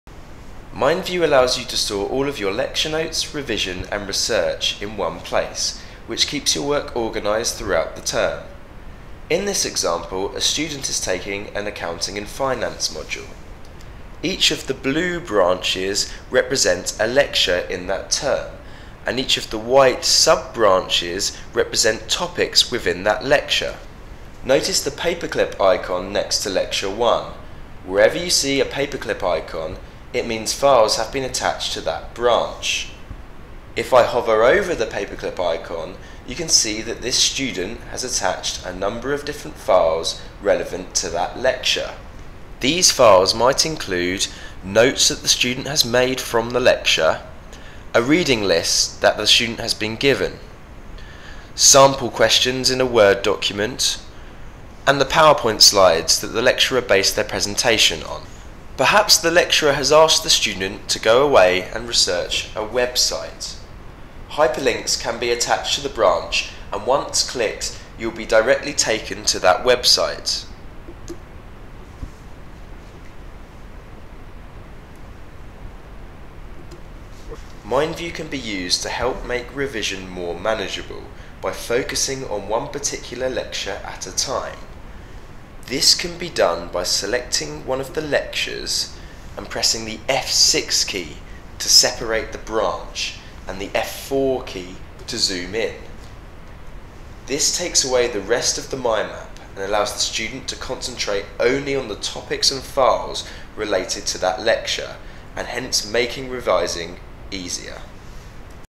MindView 5 - MindView as an Organizational Tool
This MindView 5 Video is designed to help assessors demonstrate how students can use MindView to organise their work when studying.

MindView allows students to  organise their lectures notes, revision, and research all one central mind-map making it easier to locate files.